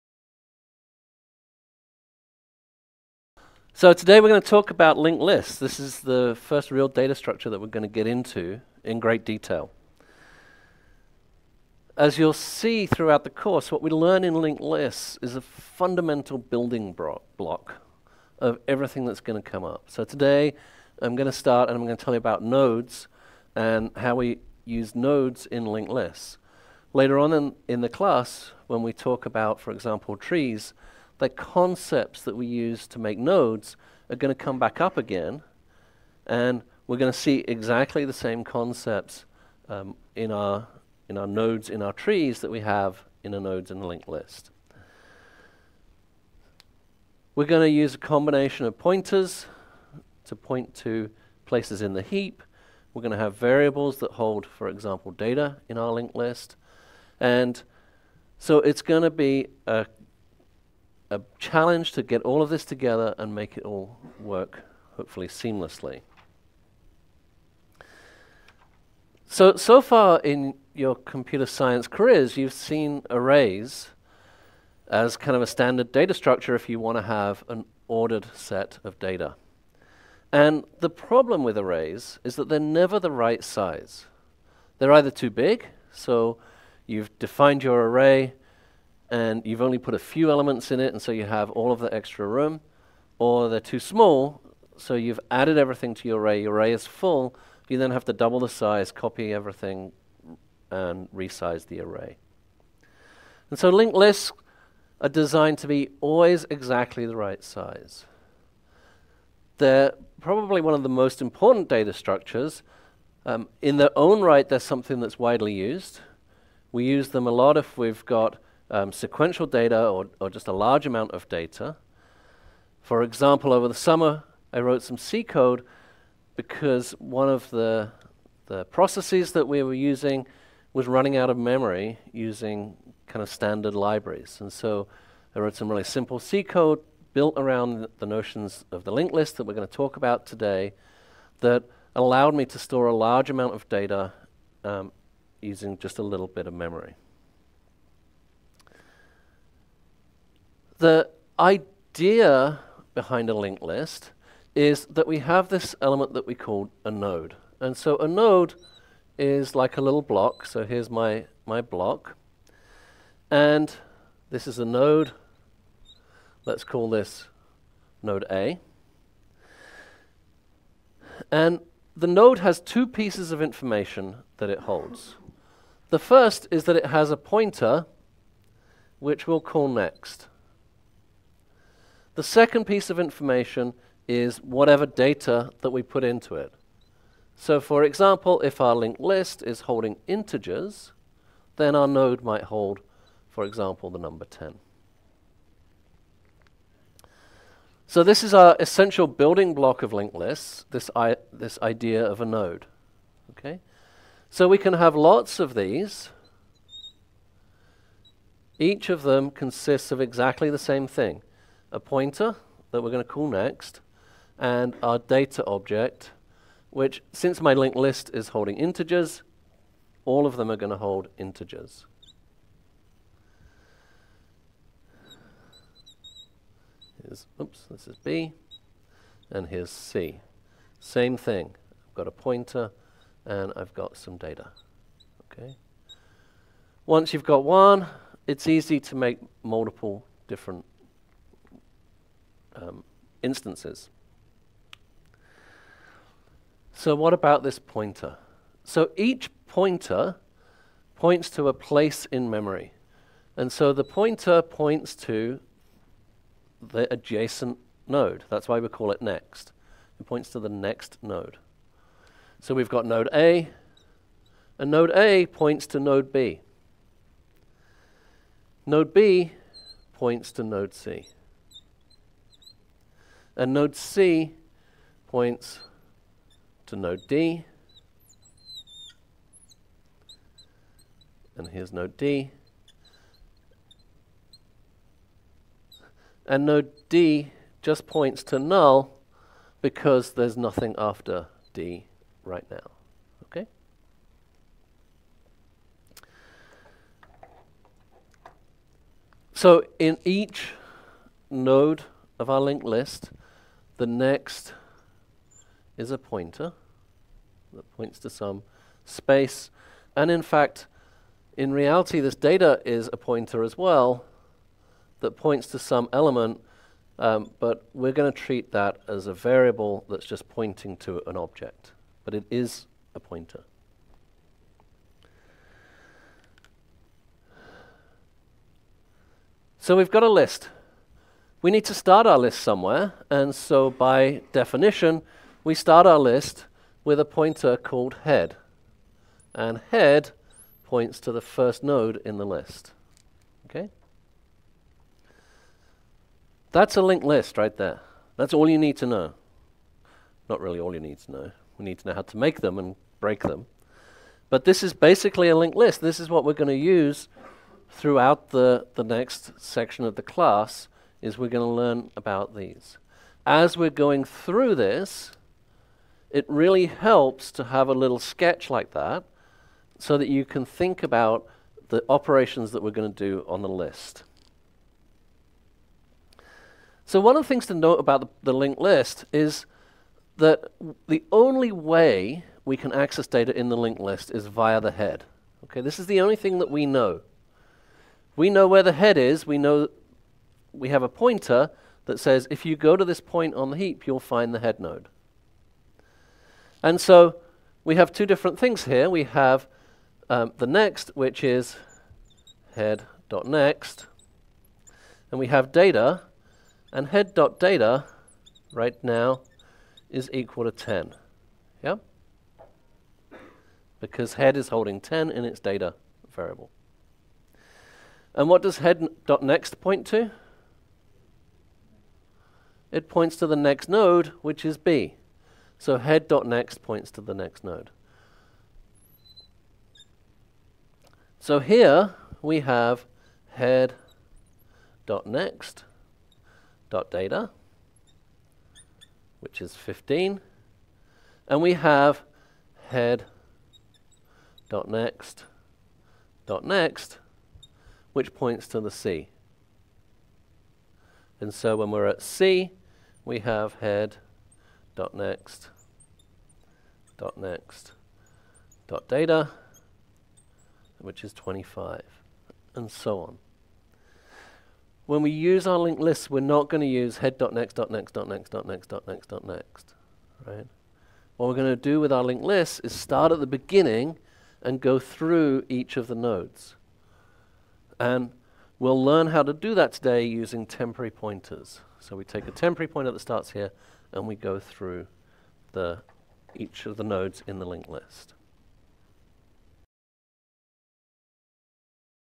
LinkedList 1 Introduction
Dr. Rob Edwards from San Diego State University introduces Linked Lists, the most fundamental data structure